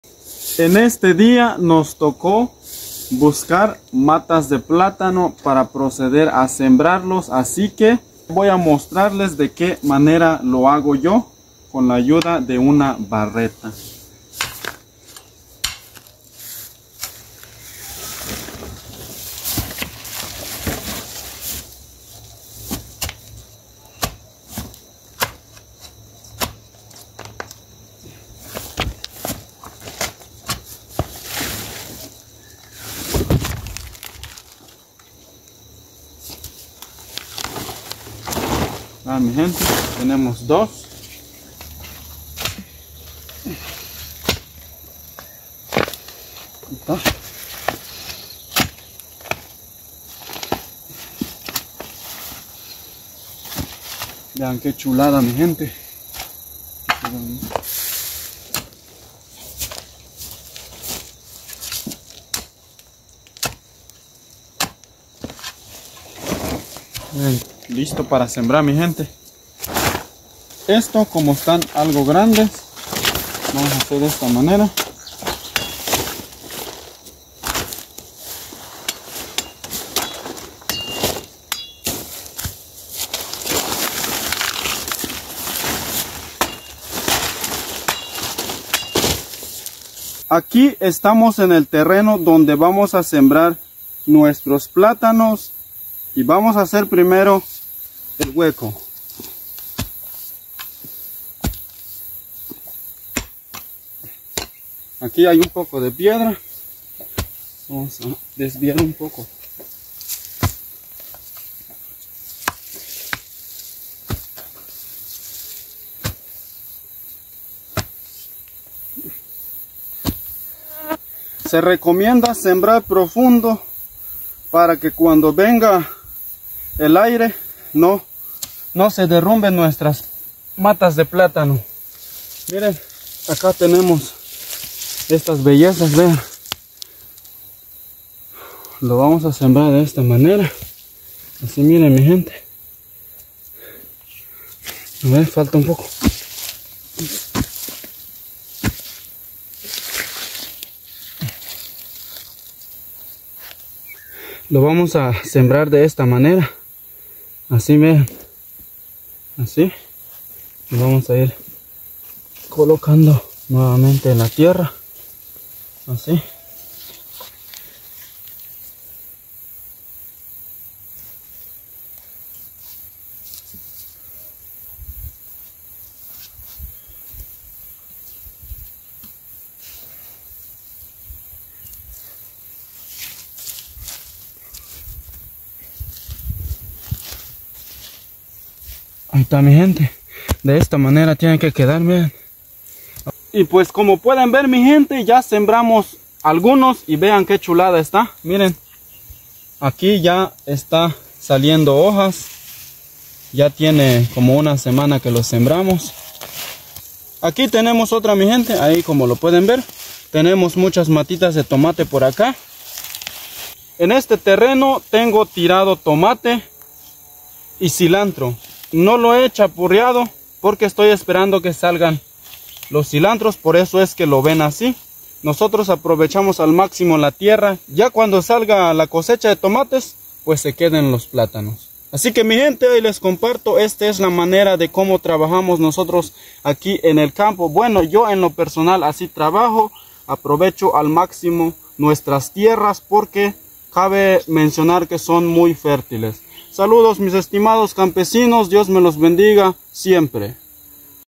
Como sembrar platanos
en este video les enseño como sebrar o plantar platanos paso a paso algunos tips y consejos para un mejor resultado del trabajo espero que les guste y les sirva.